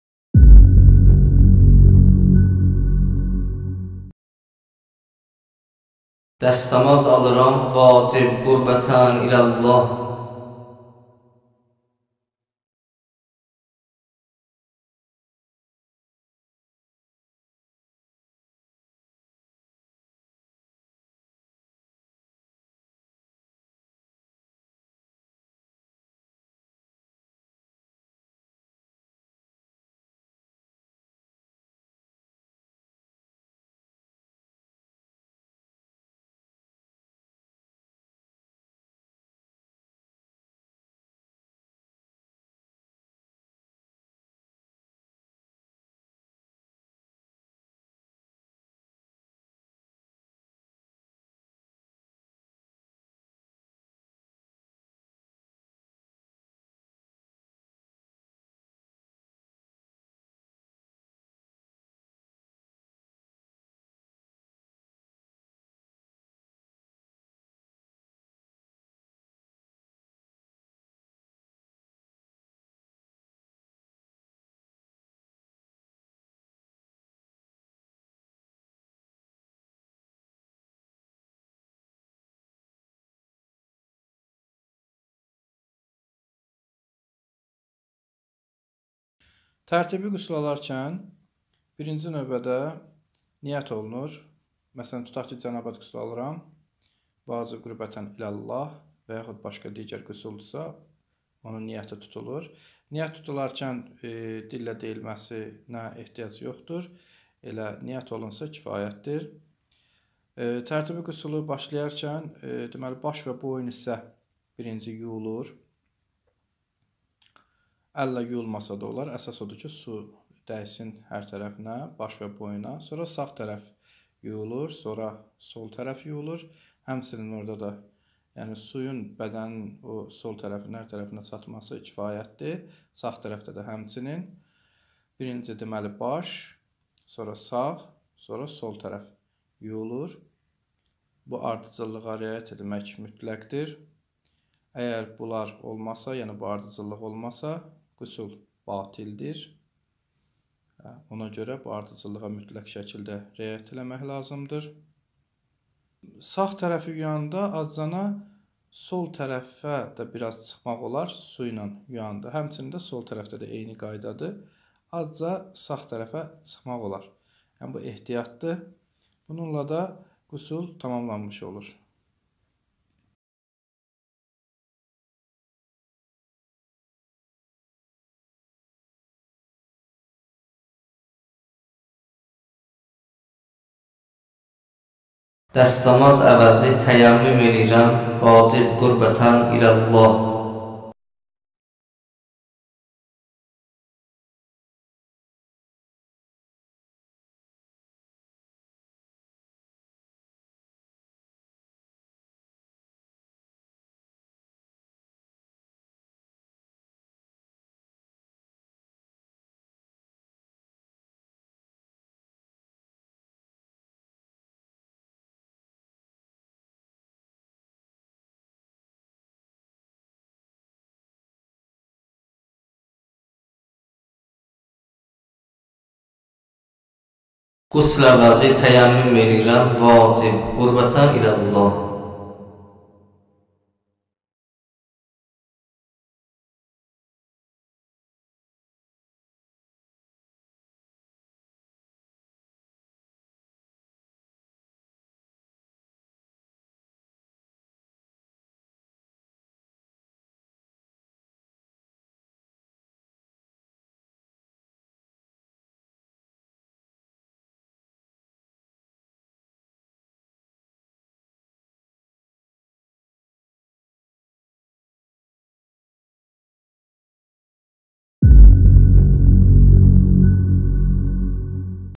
Dəstəmaz, Qüsl və Təyəmmüm - alınma qaydası
Əziz İzləyicilər! Şəriət qanunlarına rahat keçid almaq üçün (Dəstəmaz, Qüsl, Təyəmmüm) təyin olunmuş müvafiq vaxtlardan isifadə edin..
0:00 İntro (Giriş)
0:04 Dəstəmaz alınma qaydası
01:29 Qüslü alınma qaydası
02:53 Təyəmmüm alınma qaydası

Dəstəmazda ayaqlara məsh çəkmək
(Əhli-Sünnə mənbəsində).

حدثنا شعبة، قال: أخبرني عبد الملك بن ميسرة، قال: سمعت النزال بن سبرة، يقول: صلى علي رضي الله عنه الظهر في الرحبة ثم جلس في حوائج الناس حتى حضرت العصر ثم أتي بكوز من ماء فصب منه كفاً فغسل وجهه ويديه ومسح على رأسه ورجليه ثم قام فشرب فضل الماء وهو قائم ثم قال: إن ناساً يكرهون أن يشربوا وهم قيام ورأيت رسول الله صلى الله عليه وسلم فعل مثل الذي فعلت وقال: «هذا وضوء من لم يحدث»

Şöbə rəvayət etdi və dedi: Əbdülməlik bin Məysərə mənə belə dedi: Nəzzal bin Səbərənin belə dediyini eşitdim: “Əli – Allah ondan razı olsun – məsciddə zöhr namazın qıldı. Sonra əyləşib insanların problemləri ilə məşğul oldu. Ta ki, əsr vaxtı gəldi. Sonra ona bardaqda su gətirdilər. Həmin sudan ovcuna töküb üzünü və əllərini yudu. Başına və ayaqlarına məsh çəkdi”.

📚Müsnəd Əbu Davud Təyalisi, cild-1, səhifə-125, hədis 141; “Darul-hicr”, 1419/1999; təhqiq: Muhəmməd ibn Əbdülmuhsin ət-Turki.

Bizə dua etməyi və kanala abunə olmağı unutmayın.